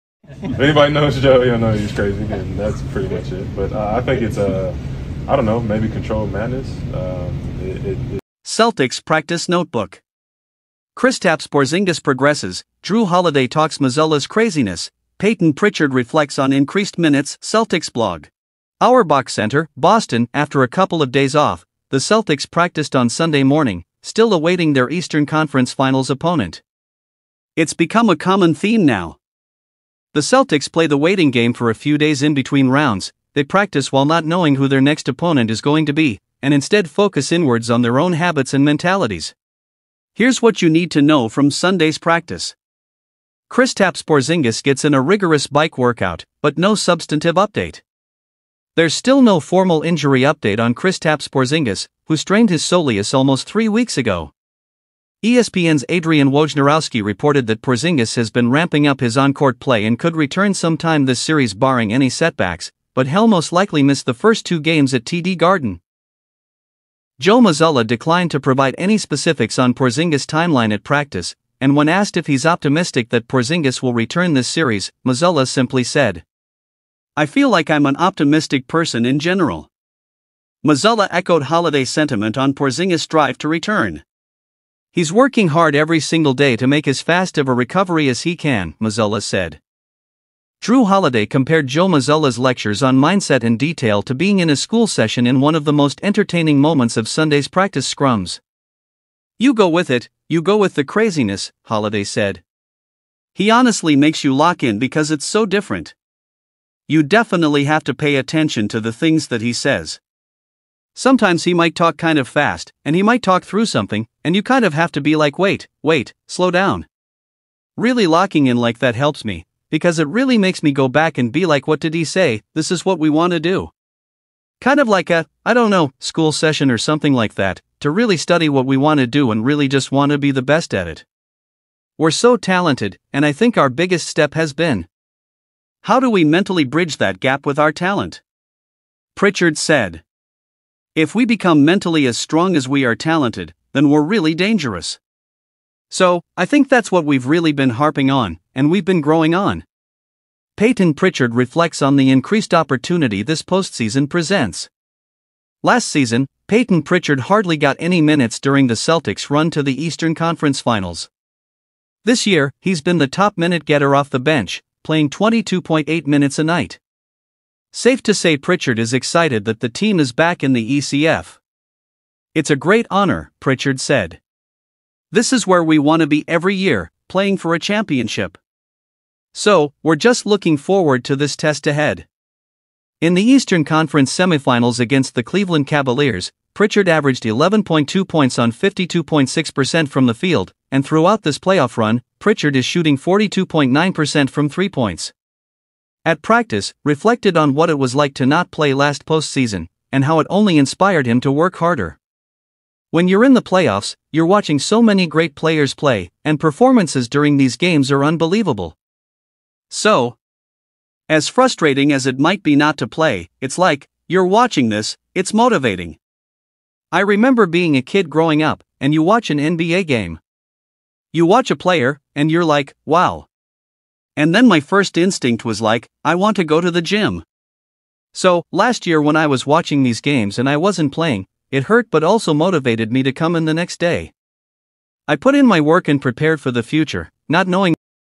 Celtics Practice Notebook: Porzingis continues to progress 💪
The Celtics Practice on Sunday after a couple of days as they await the winners of today's Pacers - Knicks Game 7 with KP working hard in a stationary bike, Jrue Holiday diving into what it's like to learn from Joe Mazzulla, and Payton Pritchard's reflecting on the progress he's made on last year playoff run.